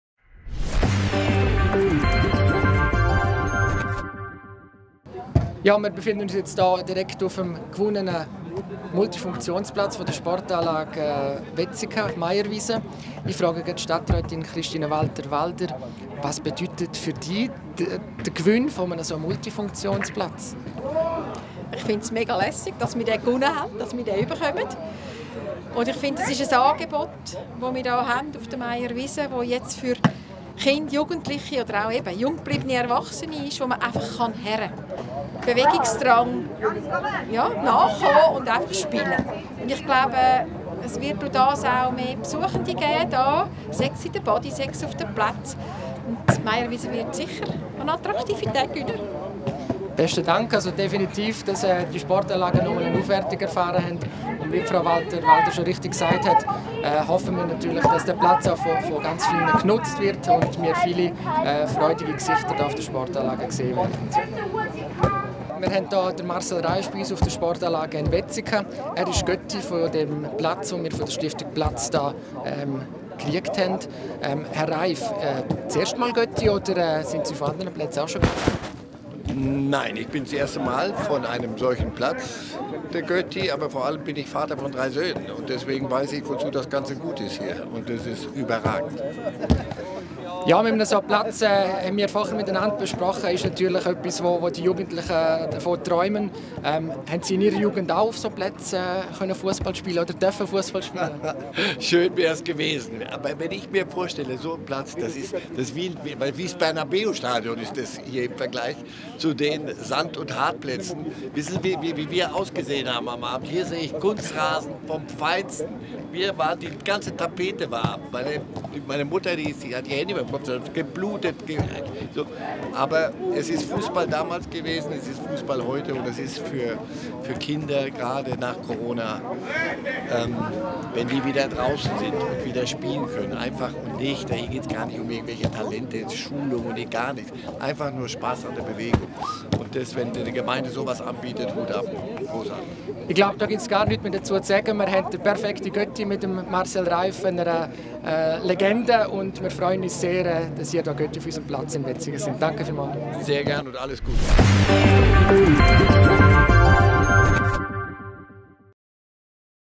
Einweihung Multifunktionsplatz Meierwiesen
Regelmässige Bewegung ist gut für die Gesundheit. Der neue multifunktionale Sportplatz von der Stiftung Platz Da auf der Sportanlage Meierwiesen unterstützt dies ideal und ergänzt das bestehende Sportangebot in Wetzikon. Ob Fussball, Handball, Basketball, Volleyball oder weitere Sportarten, der Sportplatz kann flexibel genutzt werden. Die Stiftung Platz Da verschenkt jedes Jahr seit 2016 einer Gemeinde in der Schweiz einen Multifunktionsplatz und bezweckt damit, ein Spiel-, Sport und Bewegungsanagebot für Kinder und Jugendliche zu schaffen. 2023
wurde die Stadt Wetzikon ausgelost und erfreut sich sehr an diesem neuen gewonnenen Platz. Am 5. Juli 2023 wurde der Sportplatz feierlich eingeweiht.

Produktion:
Mediaktion GmbH
Eichstrasse 6
8620 Wetzikon